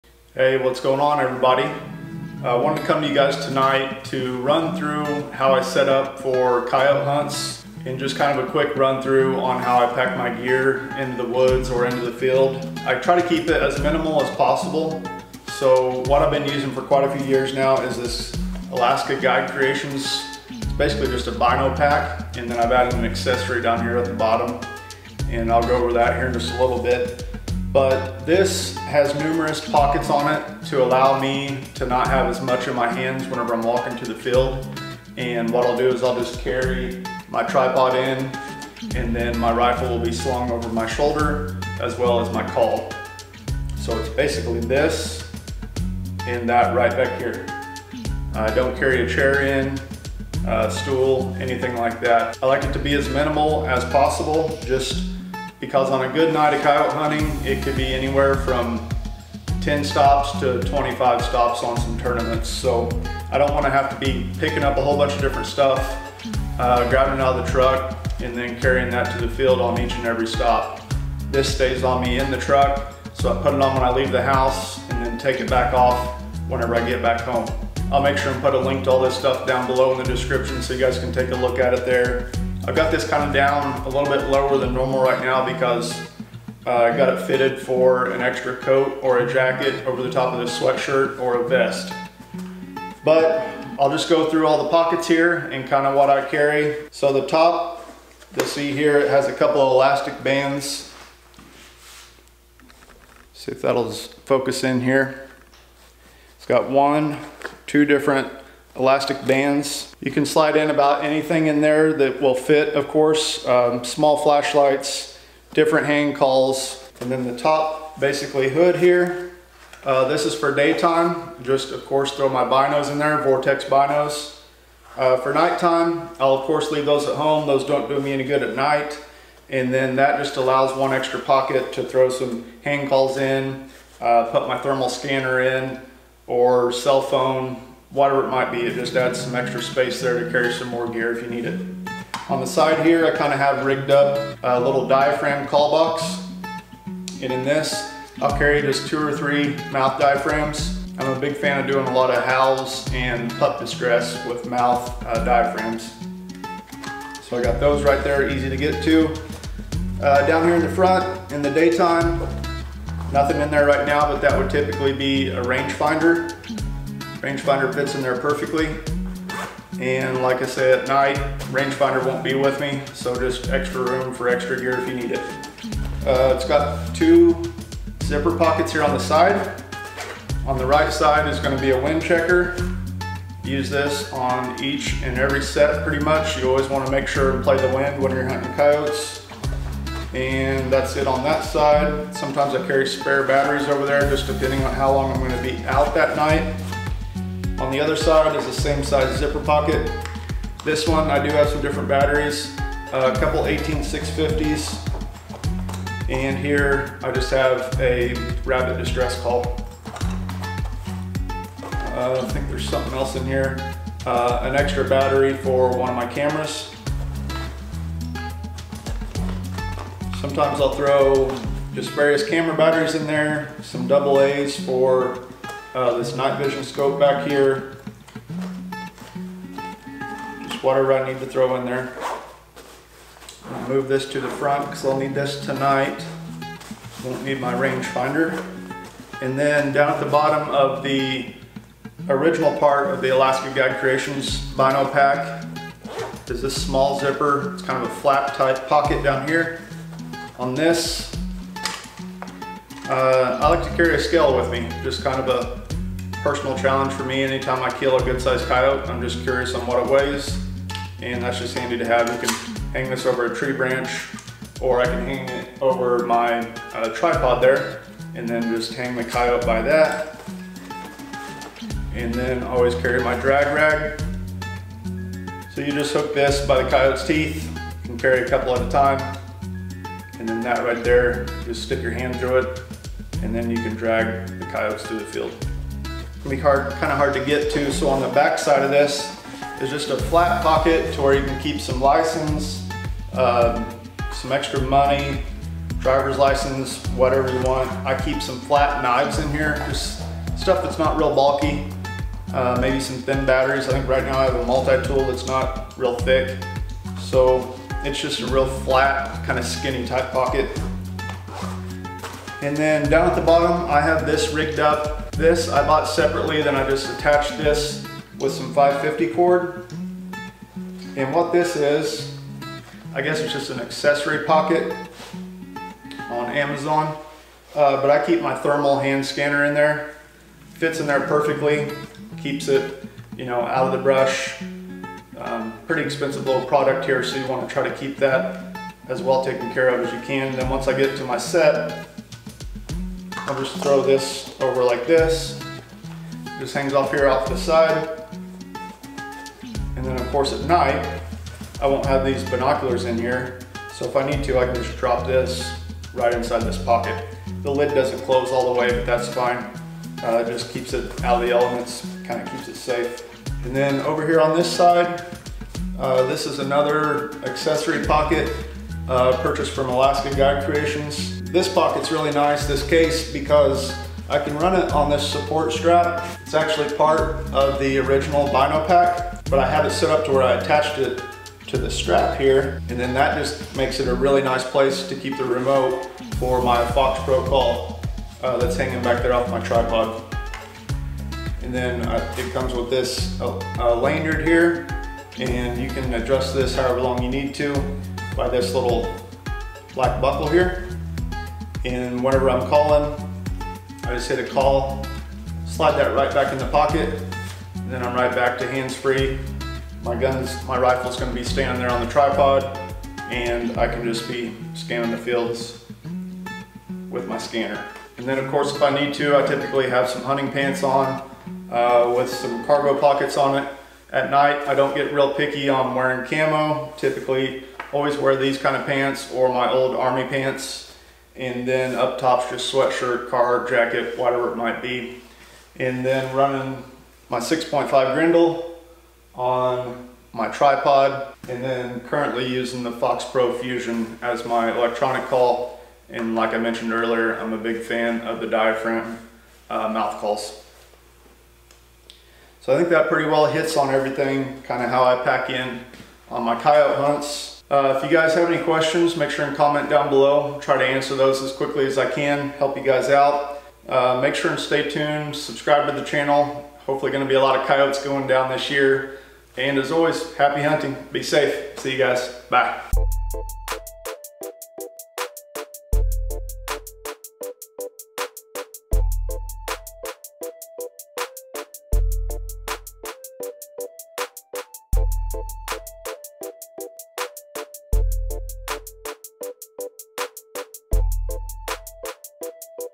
My Coyote Hunting Gear and Setup | Alaska Guide Creations
In this video I go over the gear and setup that I use for coyote hunting.  This is the gear that I use for both day and night hunting coyotes. The Alaska Guide Creations Bino Pack allows me to store most of my coyote hunting gear in it.

Alaska Guide Creations Bino Pack:

Accessory Pouch:

►PRODUCTS THAT I USE:

Main Camera
Camera Lens
Camera Lens 2
Secondary Camera
My GoPro
My Drone
My Vlogging Tripod